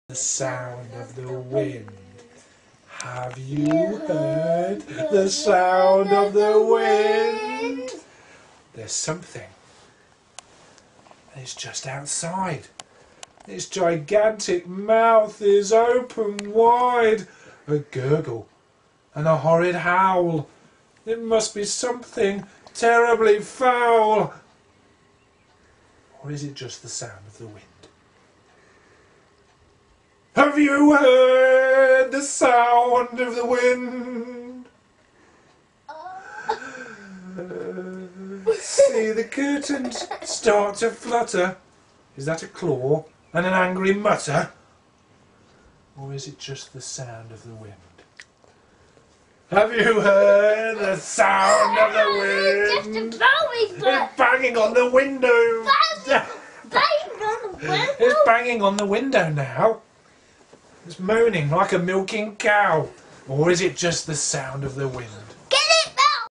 Story telling -Deep Dark Tales' -"The Sound Of The Wind"
Every new story needs to be tested so here I am testing out a new story 'The Sound Of The Wind' from the 'Deep Dark Tales' series with my 4 year old, Josh.
This is a section from the middle of the book.
Also tested it out at the local nursery today which went down..loudly! Good fun. Will probably be out ...when its all finished of course...